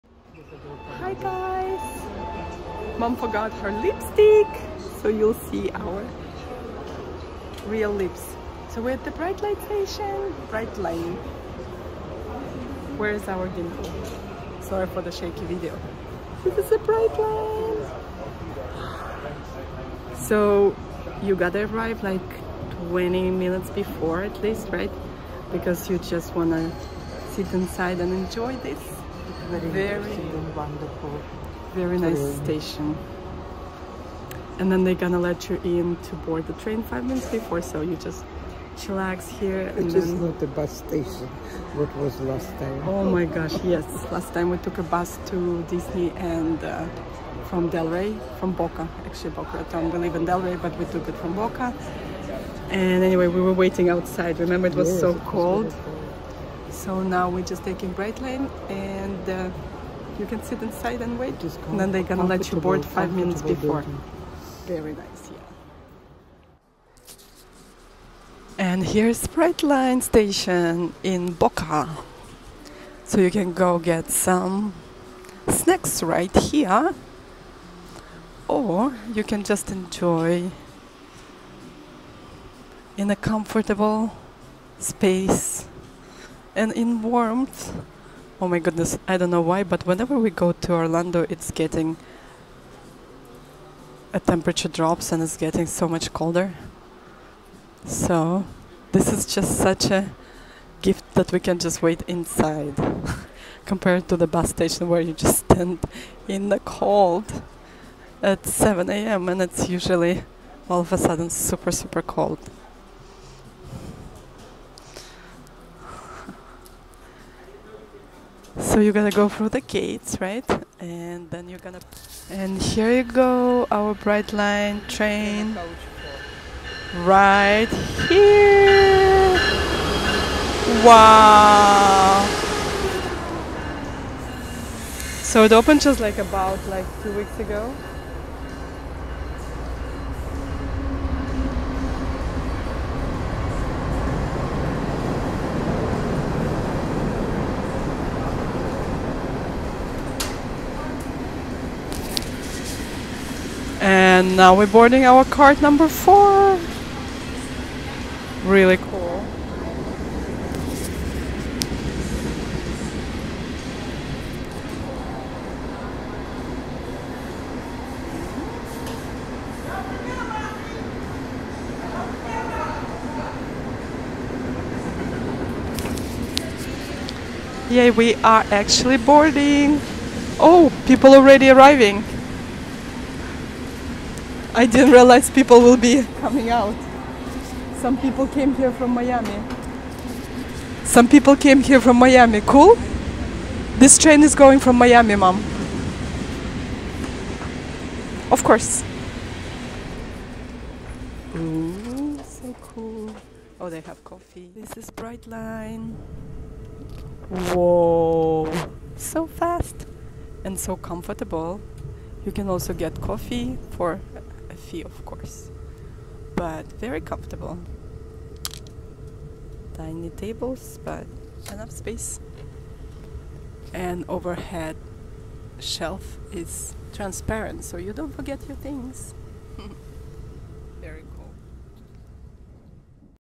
Brightline train from Miami to Orlando stops in Aventura, Fort Lauderdale, Boca Raton and Palm Beach
We took a newly opened Brightline train that takes you from Miami to Orlando with stops in Aventura, Fort Lauderdale, Boca Raton and Palm Beach! Enjoy the ride!